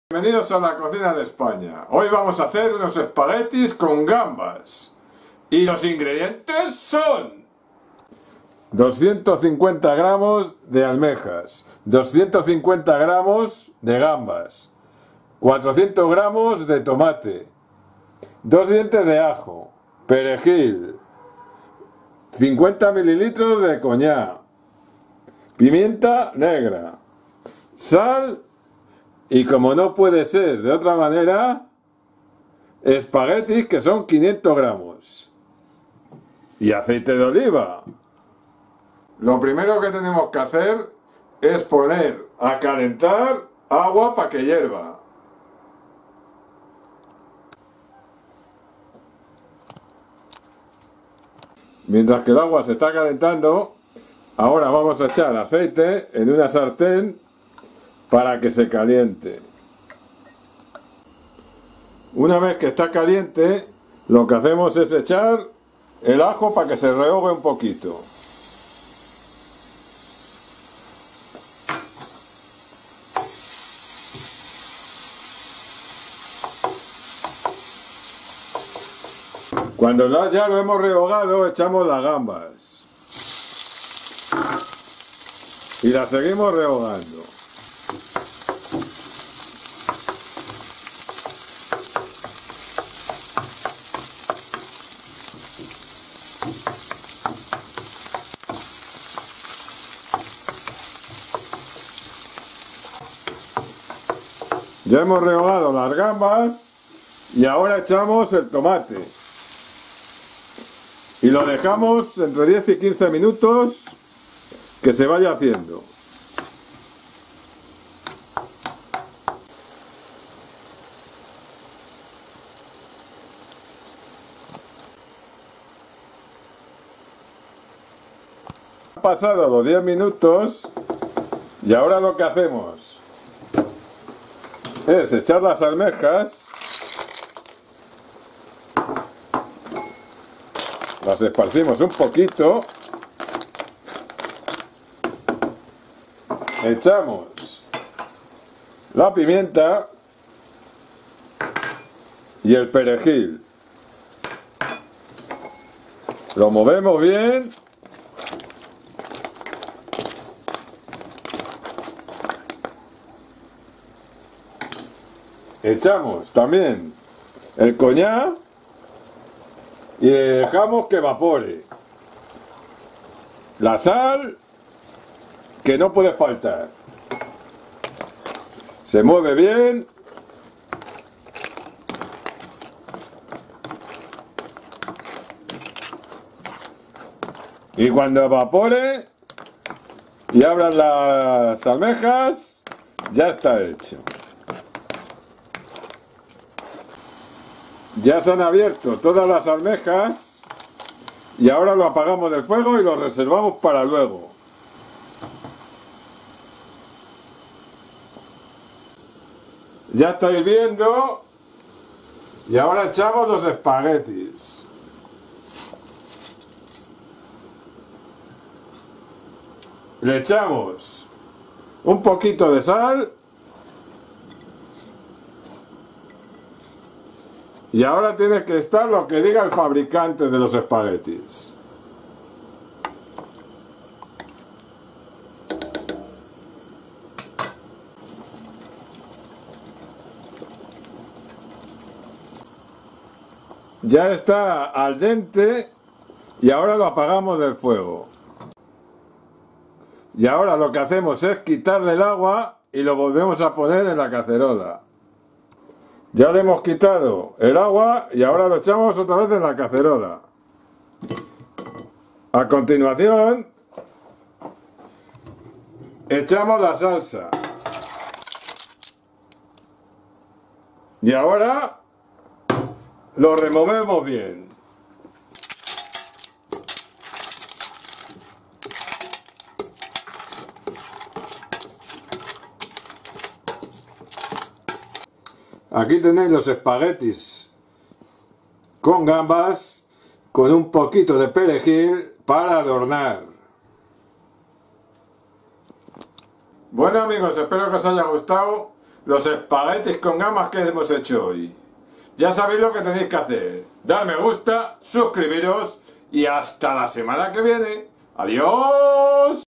ESPAGUETIS CON GAMBAS
Hola amigos bienvenidos a "La cocina de España". En el video de hoy vamos a hacer una receta de espaguetis con gambas, es una receta fácil y rápida de hacer.
Aquí os dejo los ingredientes de la receta de espaguetis con gamba:
250 gramos de almejas, 250 gramos de gambas, 400 gramos de tomate, 2 dientes de ajo, perejil, 50 mililitros de coñac,  pimienta negra, sal,  500  gramos de espagueti  y aceite de oliva